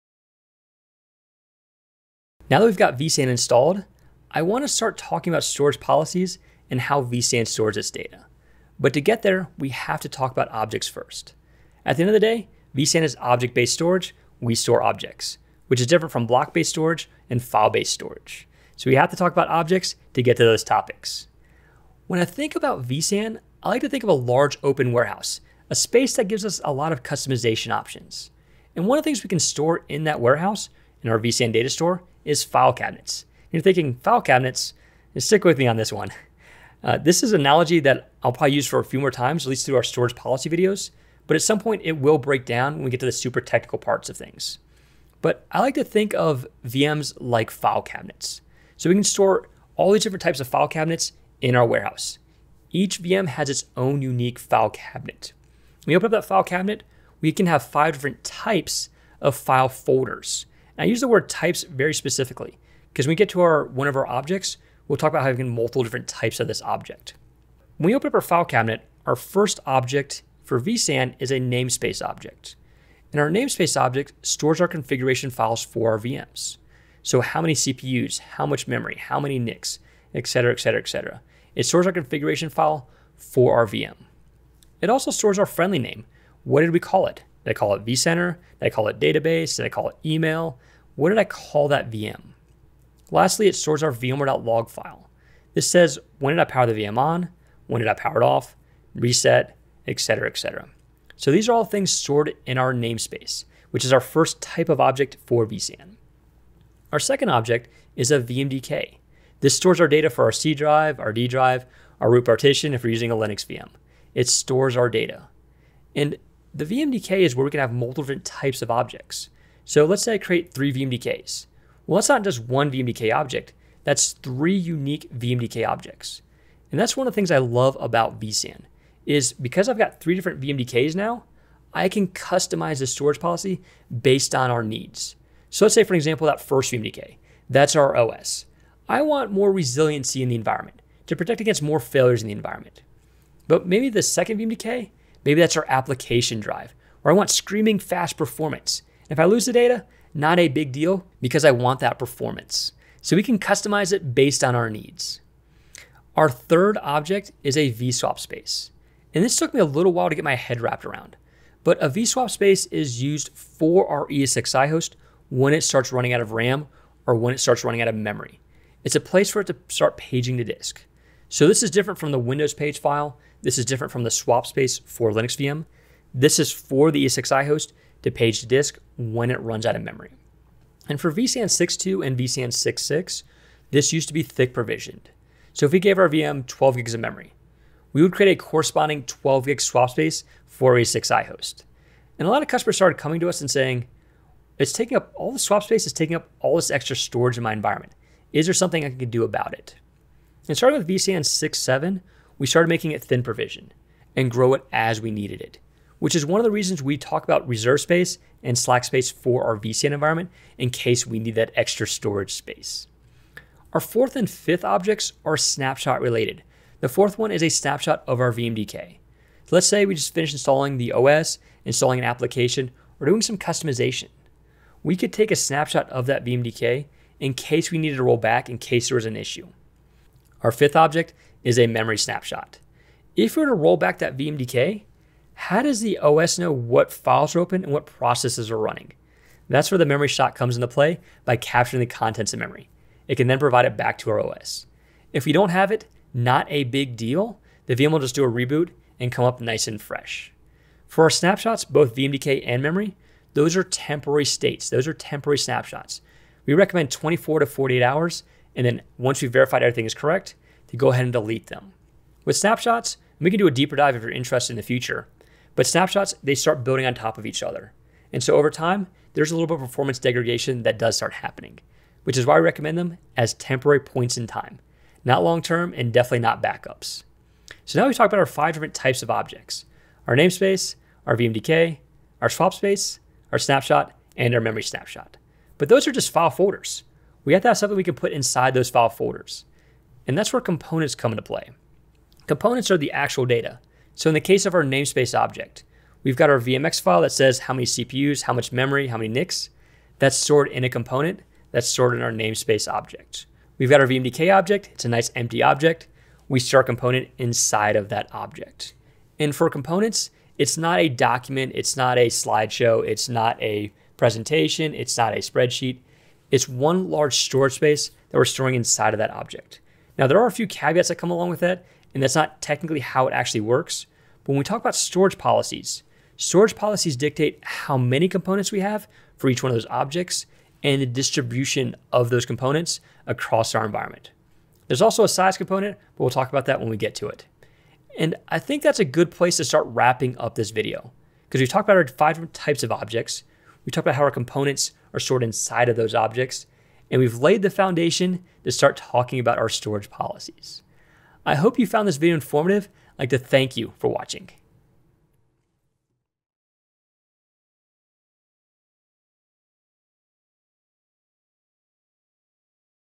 What are vSAN Objects?
vSAN is object-based storage, which is different from file-based storage and block-based storage. This video walks through the five different types of objects in vSAN.
-Namespace
-VMDK
-vSWP
-Snapshot
-Memory Snapshot